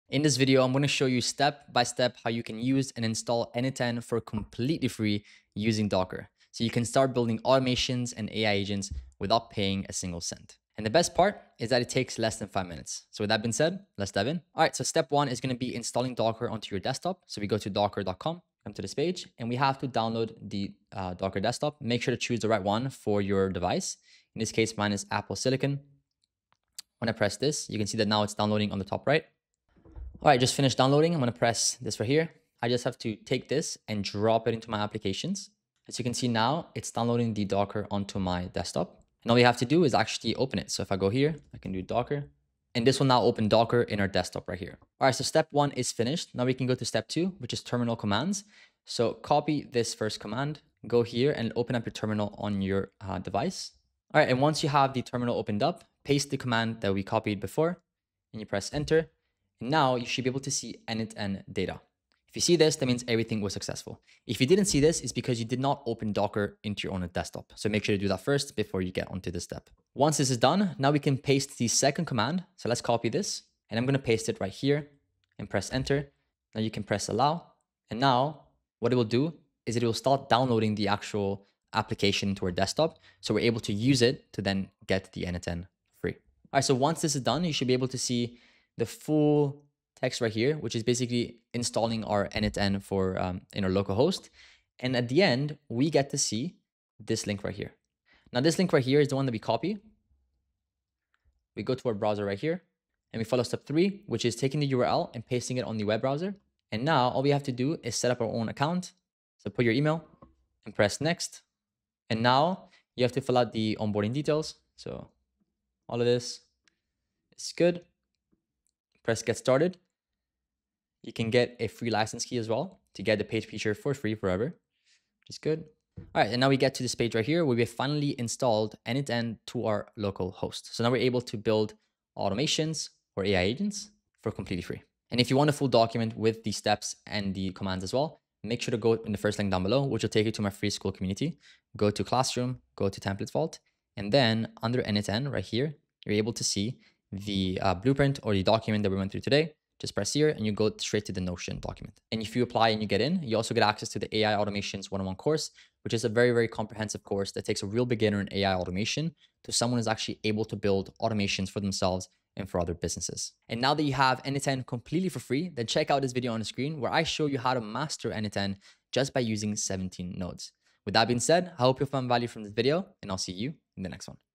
How To Self-Host N8N For FREE (In 3 Minutes)
Get my 1-1 support to Start and Scale your AI Agency

In this video, I show you the step-by-step process to install and run n8n on your own computer for free using Docker. Unlock unlimited workflows and executions without ever paying a monthly fee, giving you the full power of enterprise-grade automation right on your local machine.

We'll cover:

- How to download and install Docker Desktop on your Mac or Windows computer.
- The two simple terminal commands you need to create a persistent data volume and run the n8n container.
- How to access your new local n8n instance and set up your owner account for the first time.

Timestamps
00:00 - Intro
00:13 - Step 1
00:47 - Step 2
00:58 - First Command
01:11 - Second Command
01:32 - Step 3
01:46 - Setting Up Your n8n
02:15 - Outro